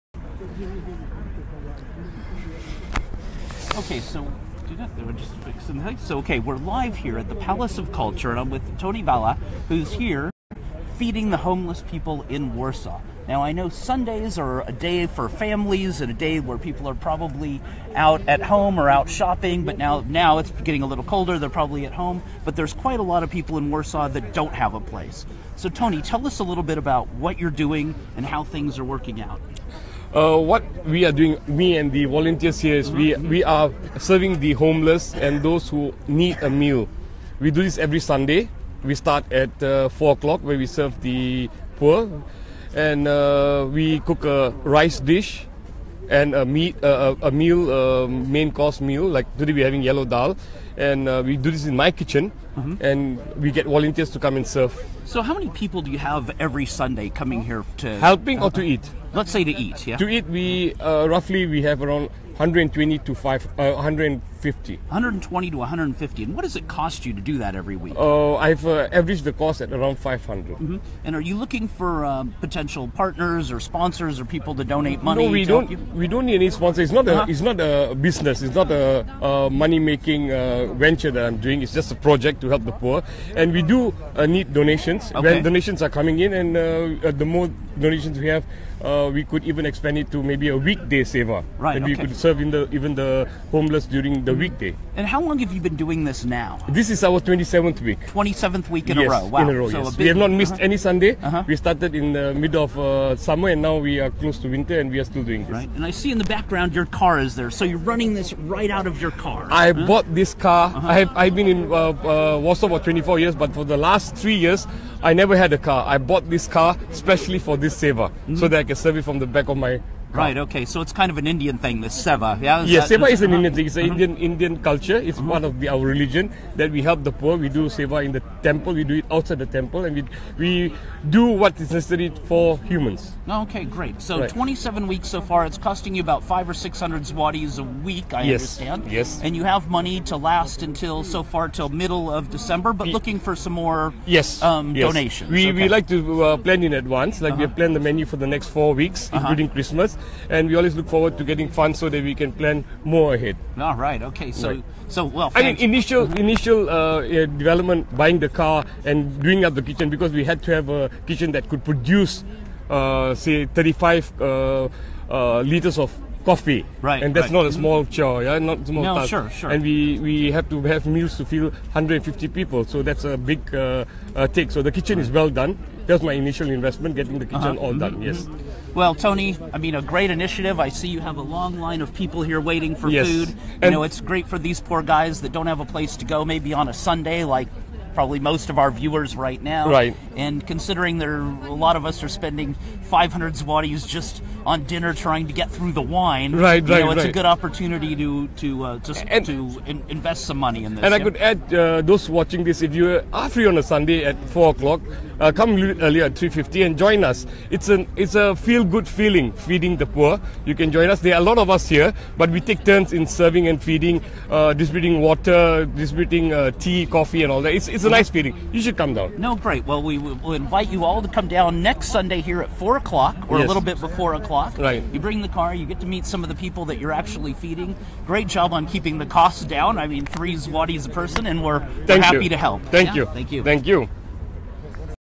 Toni Walia feeding the homeless in Warsaw, Poland at the Palace of Culture
Toni Walia feeding the homeless in Warsaw, Poland at the "Palace of Culture" (every Sunday 16:00) Toni along with Andrew Peter Eddles and several other expats and Polish are really heros to some of the homeless here in Warsaw supplying good Indian food and from Andrew clothes and some other essentials - for donations contact Toni or Andrew - if you can't reach them shoot me an email and I'll forward it to them